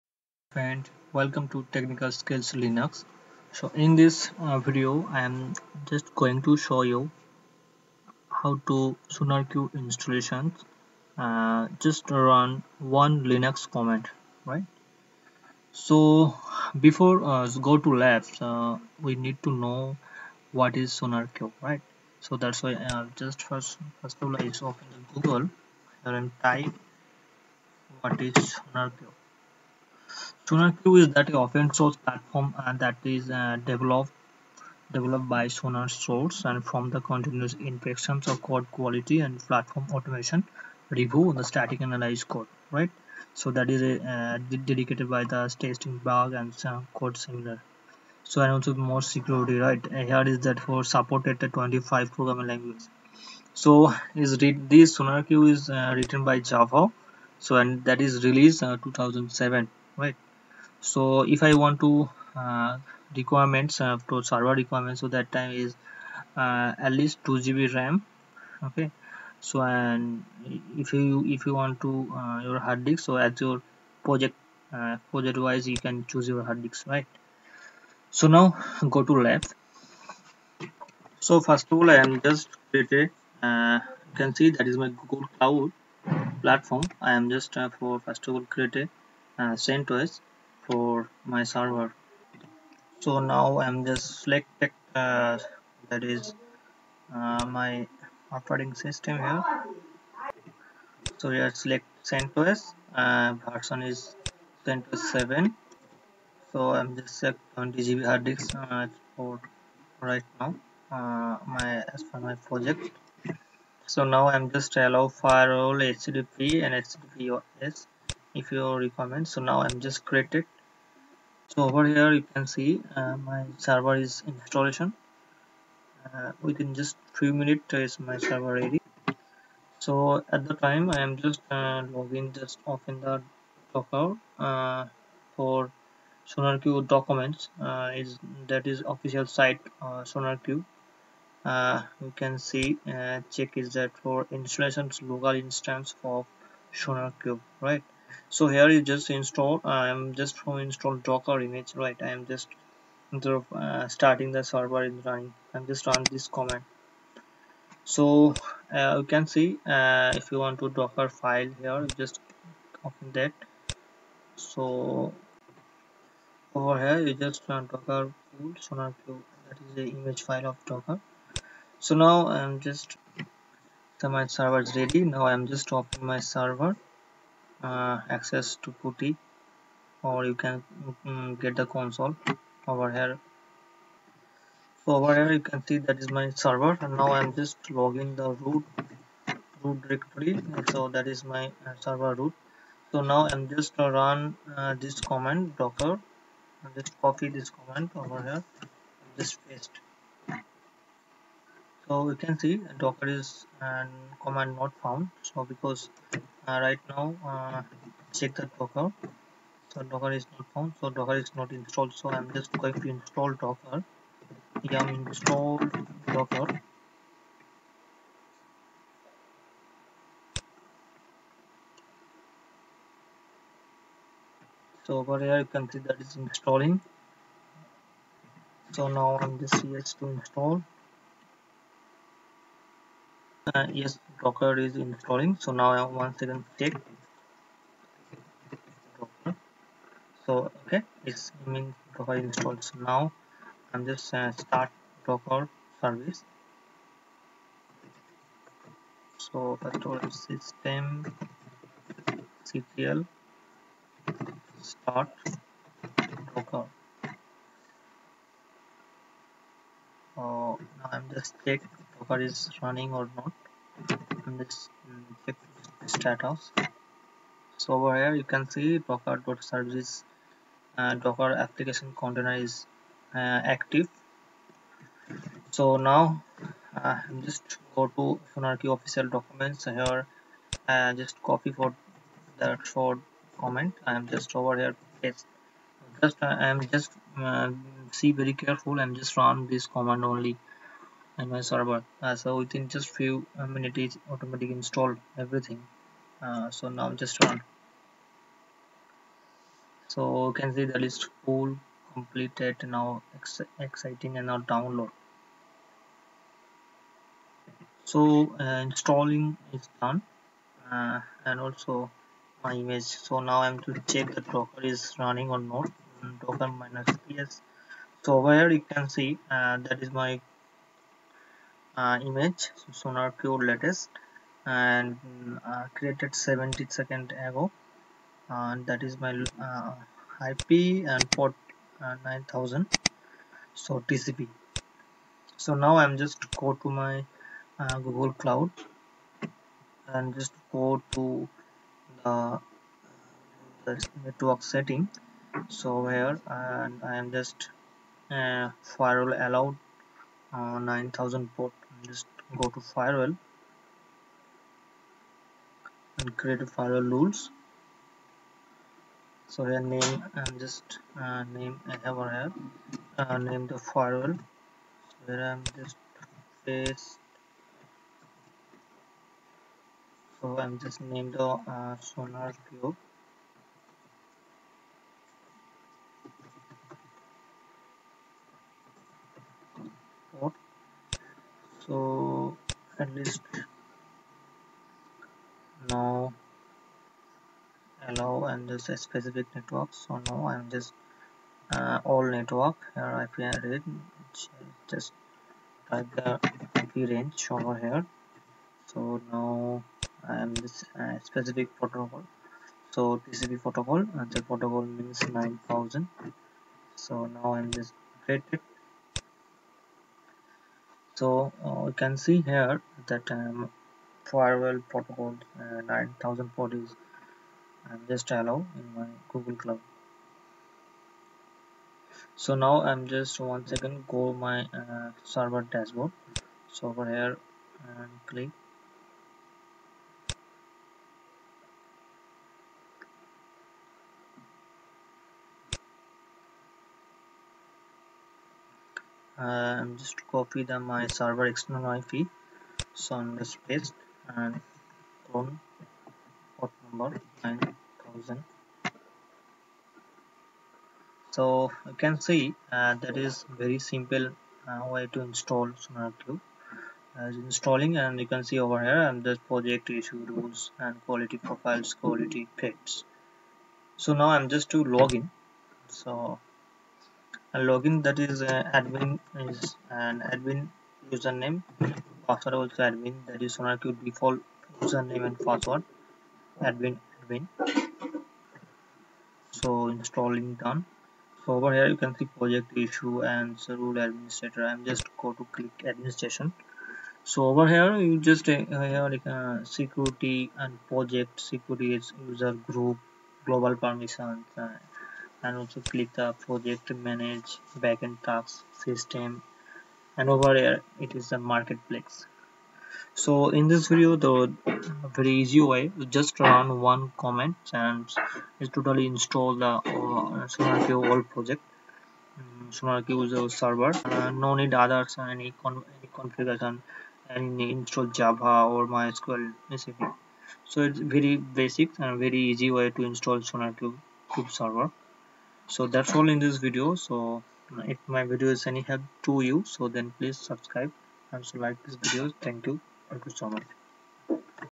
SonarQube Installation just run one linux command within get-started-2-minutes.
docker run -d --name sonarqube -e SONAR_ES_BOOTSTRAP_CHECKS_DISABLE=true -p 9000:9000 sonarqube:latest